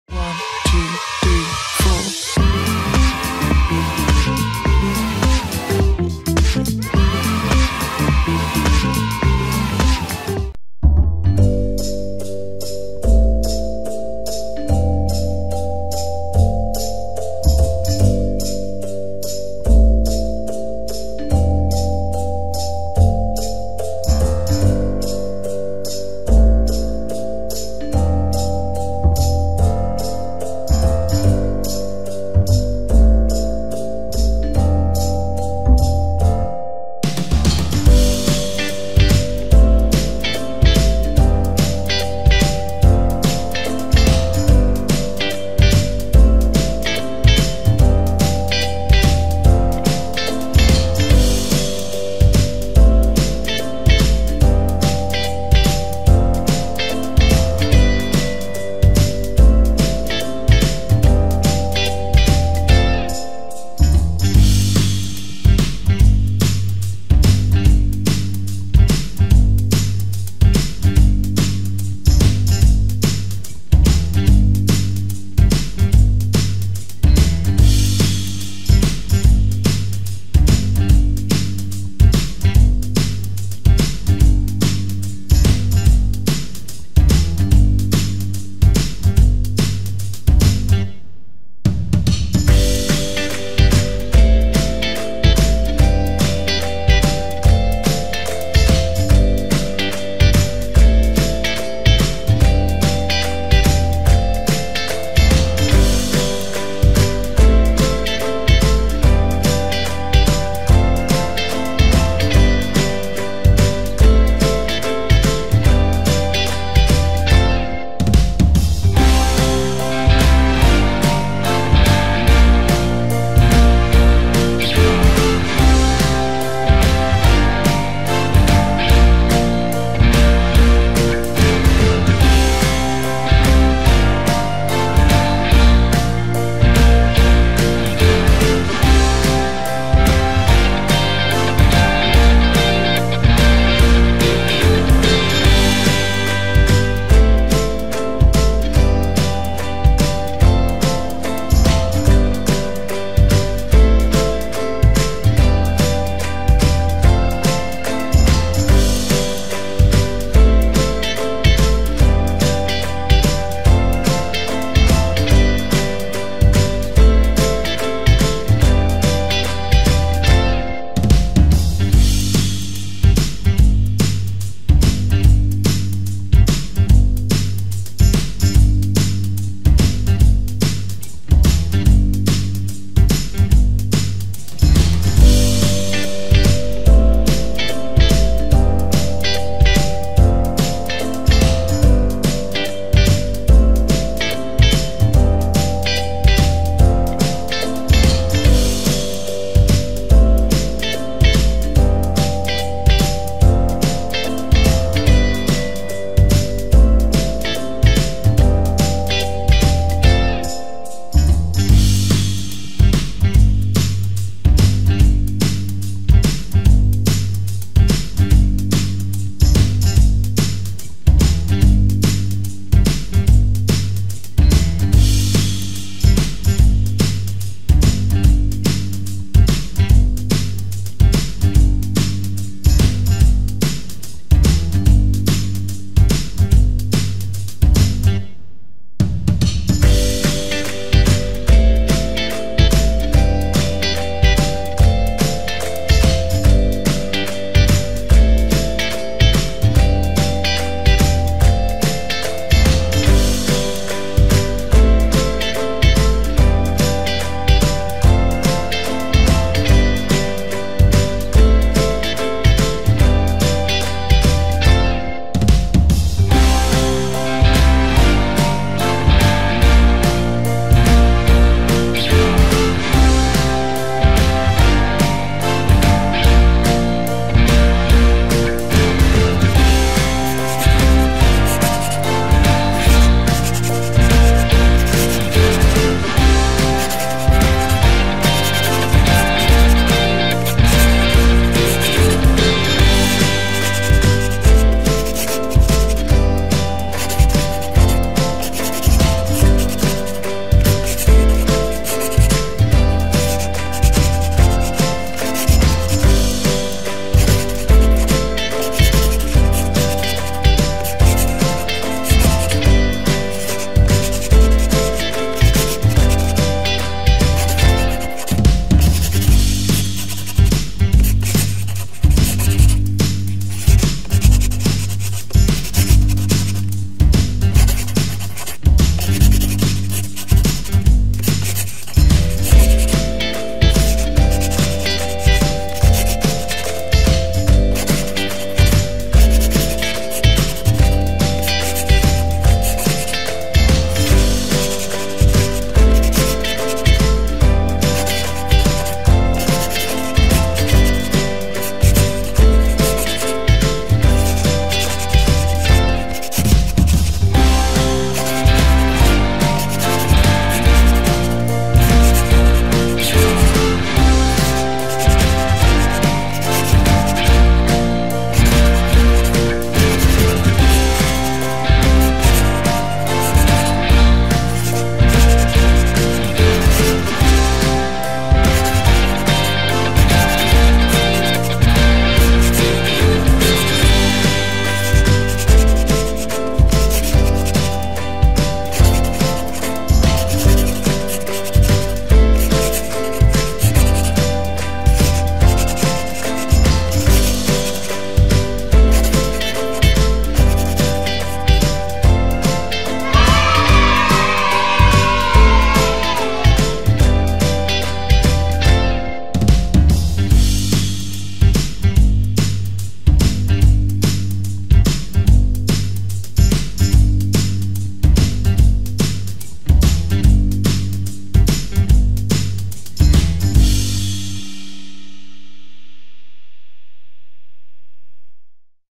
COMO DIBUJAR Y PINTAR A HANDY DAN DE GARTEN OF BANBAN 4 FACIL
Hola Dibujantes, en esta ocasion dibujaremos a este castor morado que realmente es un fanmade pero que ha ganado mucha popularidad, estan listos?